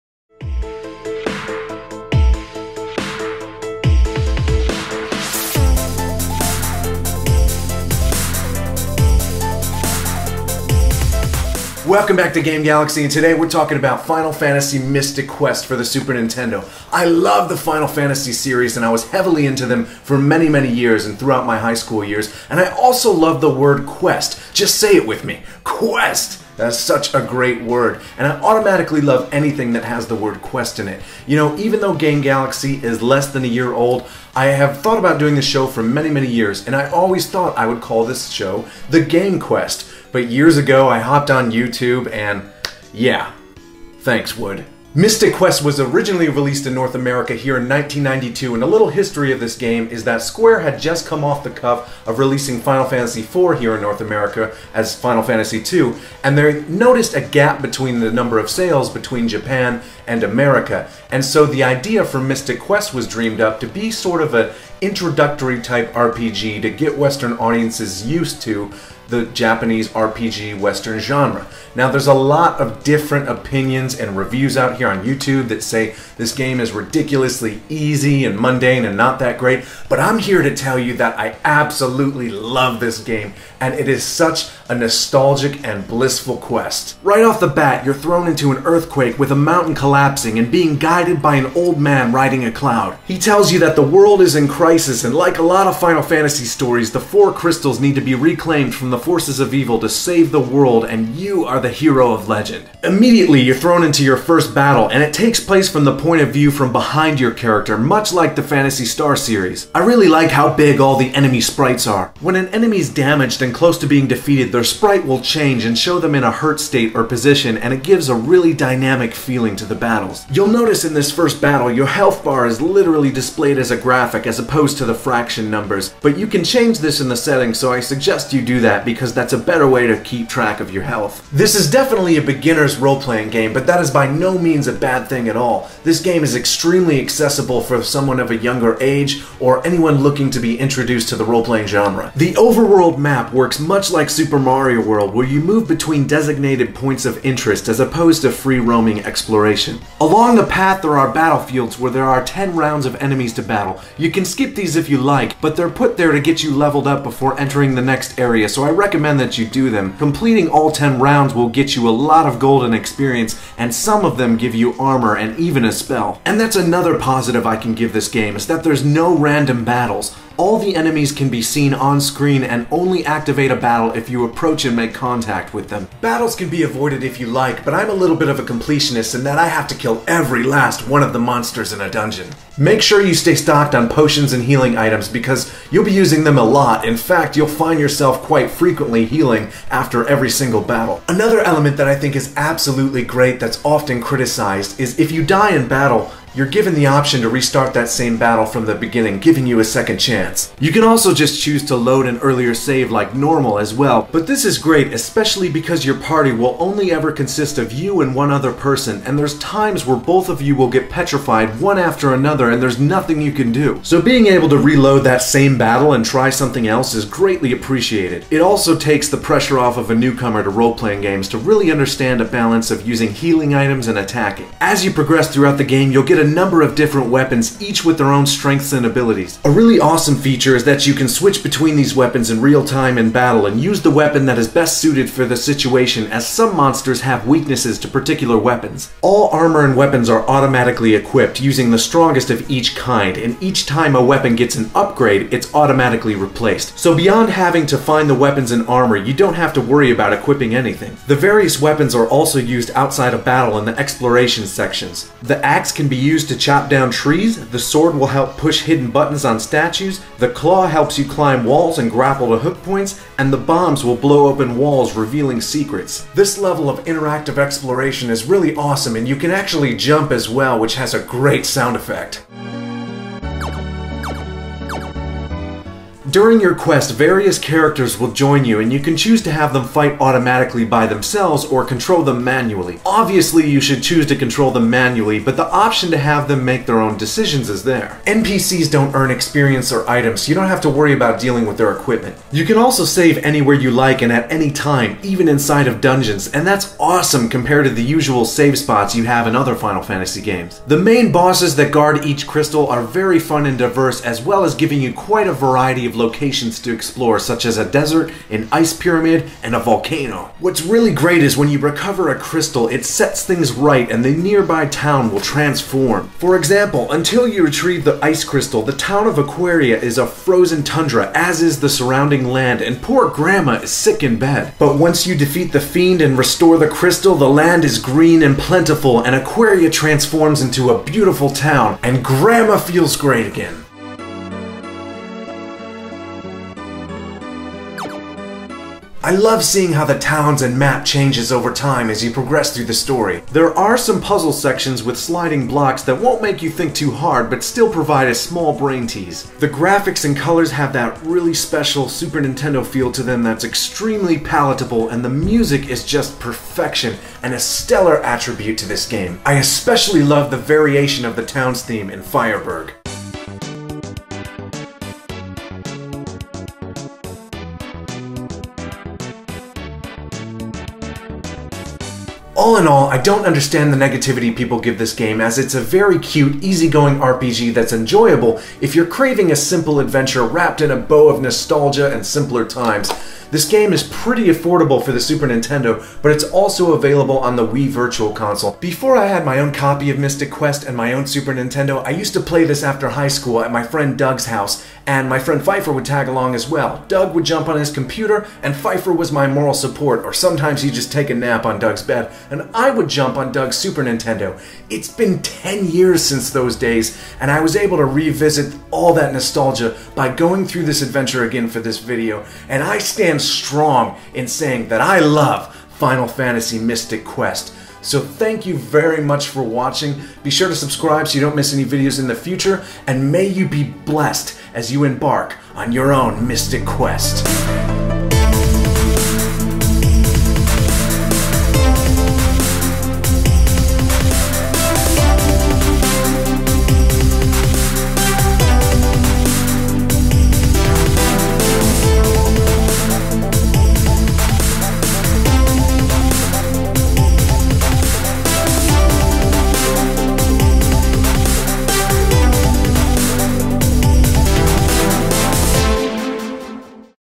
FINAL FANTASY MYSTIC QUEST REVIEW - Game Galaxy
Behold one of my favorite nostalgic Adventures! Final Fantasy Mystic Quest for the Super Nintendo! Released by Square in North America in 1992 - This is such an underrated game that is such a cute simple adventure wrapped in a bow of nostalgia and simpler times.